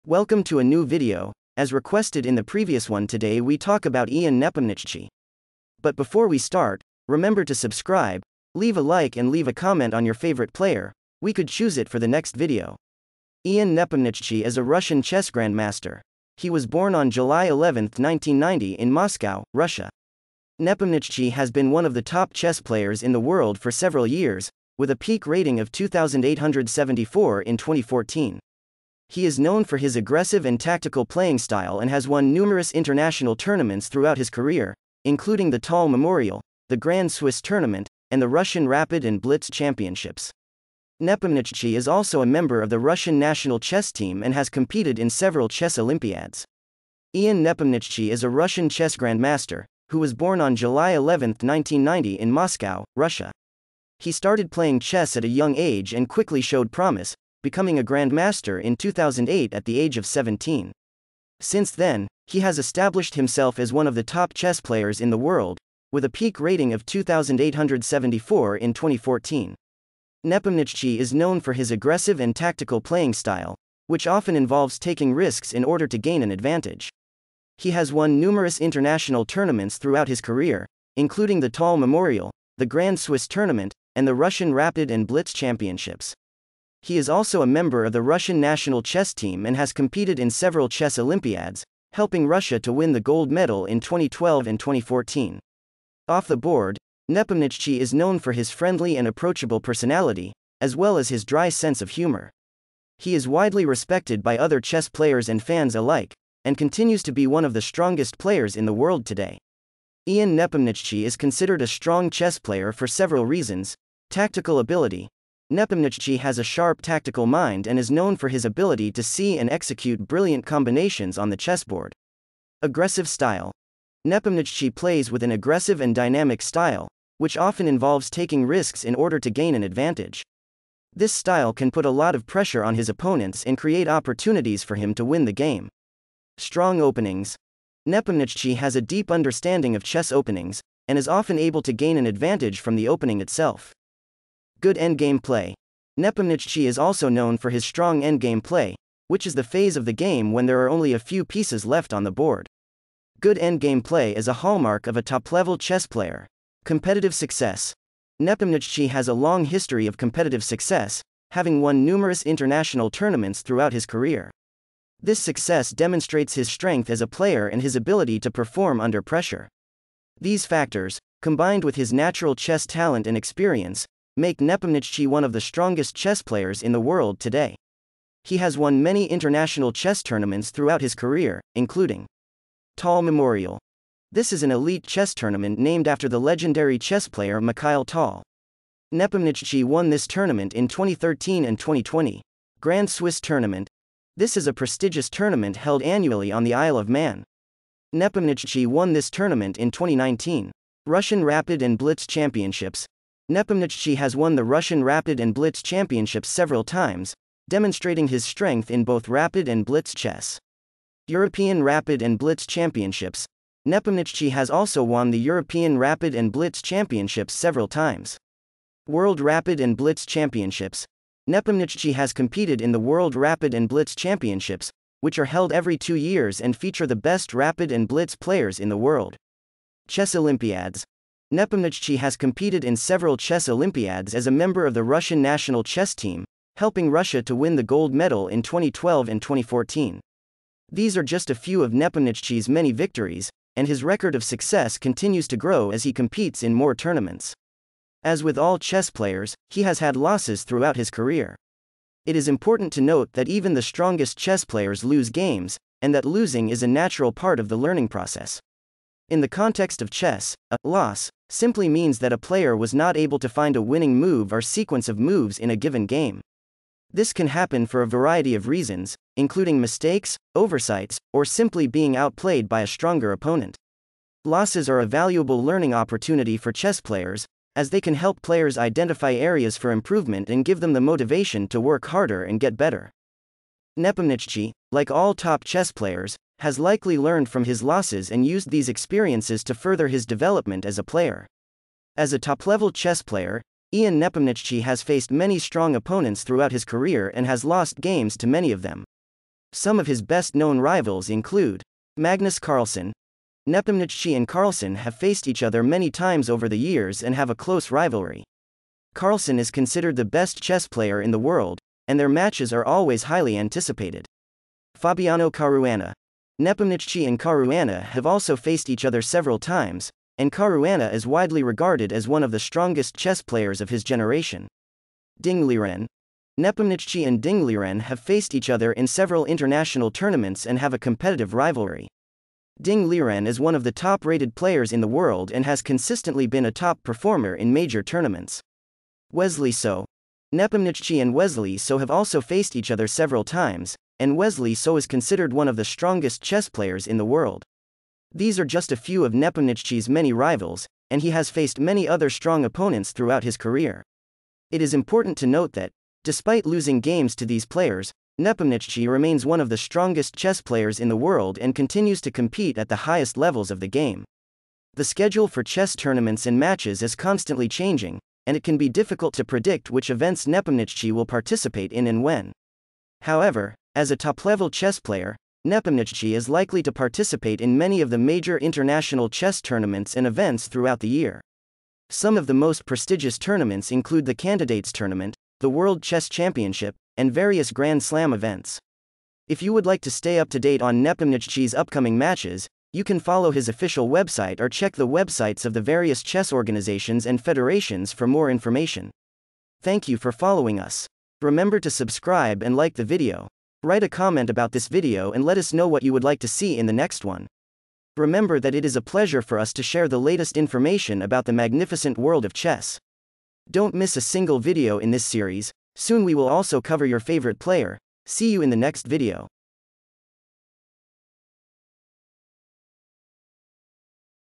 Who is Ian Nepomniachtchi and why is he ONE OF THE BEST players in the world?
Welcome to a new video about Ian Nepomniachtchi, one of the strongest and most exciting chess players in the world today. In this video, we will take a closer look at the man behind the moves and explore why he is considered a top-level player and a force to be reckoned with on the chessboard.

Born in Russia, Ian Nepomniachtchi began playing chess at a young age and quickly showed a natural talent for the game. Over the years, he has competed in numerous international tournaments and has become known for his aggressive and unorthodox playing style. Despite his relative youth, Nepomniachtchi has already achieved several significant victories and has established himself as a major player in the world of professional chess.

In this video, we will examine Nepomniachtchi's rise to the top of the chess world and take a closer look at some of his most memorable victories. We will also delve into his playing style and explore what sets him apart from other top-level chess players. Whether you are a fan of Ian Nepomniachtchi, a chess enthusiast, or simply interested in learning more about this exciting and talented player, this video is a must-see.

So, if you are ready to immerse yourself in the world of Ian Nepomniachtchi, be sure to hit the subscribe button, like the video, and leave a comment to let us know what you think. We hope you enjoy this in-depth look at one of the most promising and talented chess players of our time.